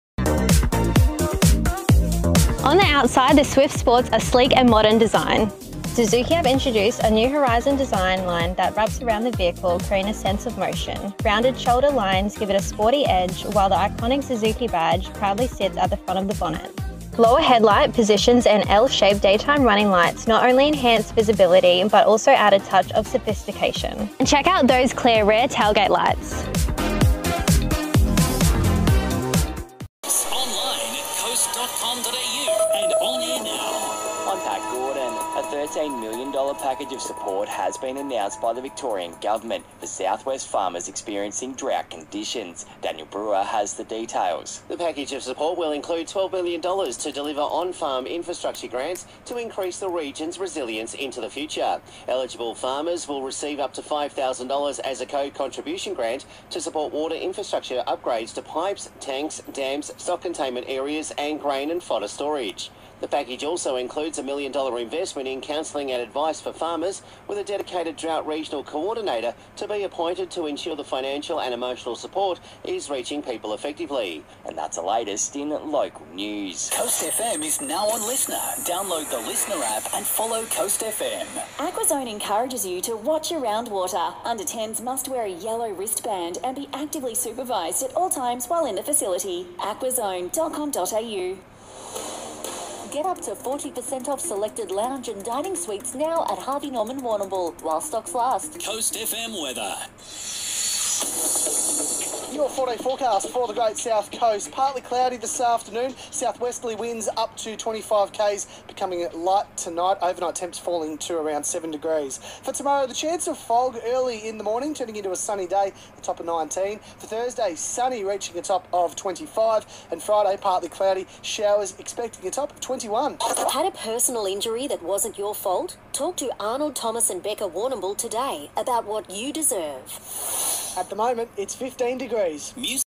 Southwest Coast 95.3CoastFM - 2PM 5News Update, Tuesday October 1st 2024
With Pat Gordon (Local News Headlines) and Lachlan O'Rooke (Weather)